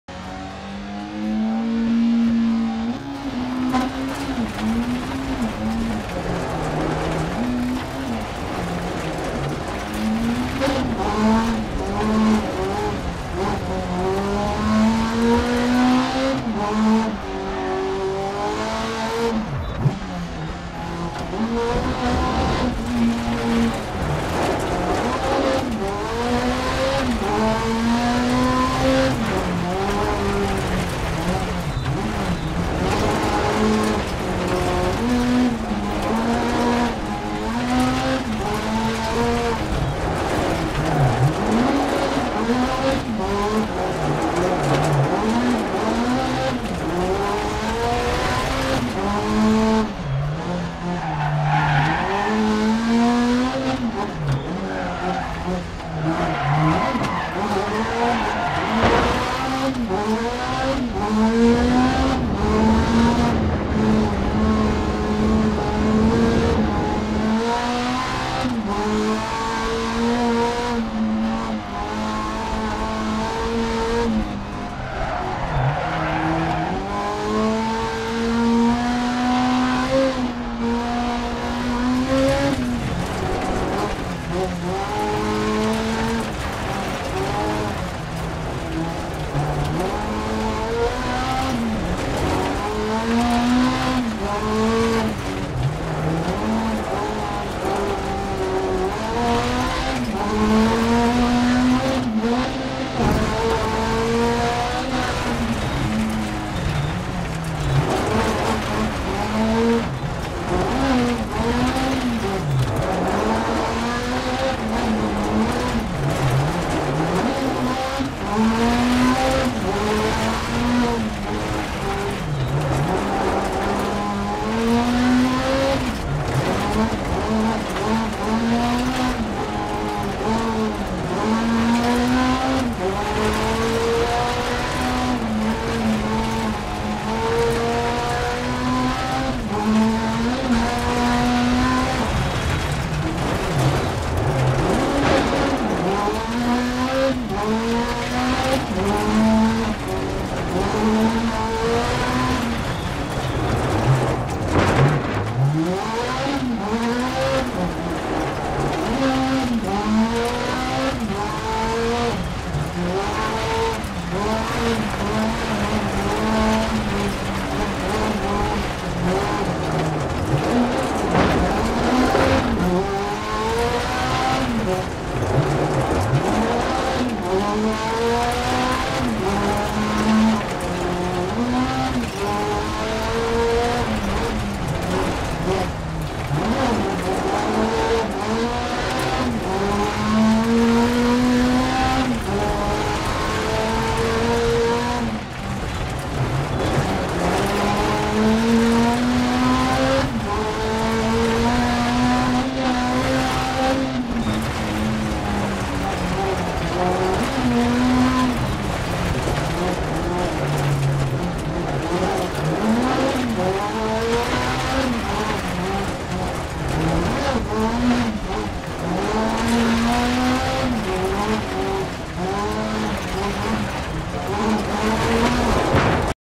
Ford RS200 Rally Australia Dash Cam
This beast sounds soo greatddd!